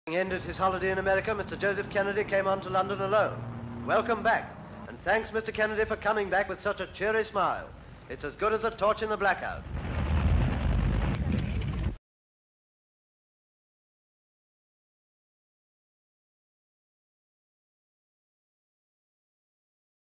Joseph Kennedy returns to England (1940)
Joseph Kennedy, United States Ambassador to the United Kingdom, arrives back in London

Full Description:

ENGLAND: London: Hounslow: Heston: Heston Aerodrome:
EXT

HESTON AIRPORT. Sumner Welles arrives. (No plane landing)

KENNEDY, Joe & Mrs J. Arrives Heston: smiles.

Personalities - Politicians

plane, airplane, aeroplane, American, Joe Kennedy
Background: Joseph Kennedy, United States Ambassador to the United Kingdom, arrives back in London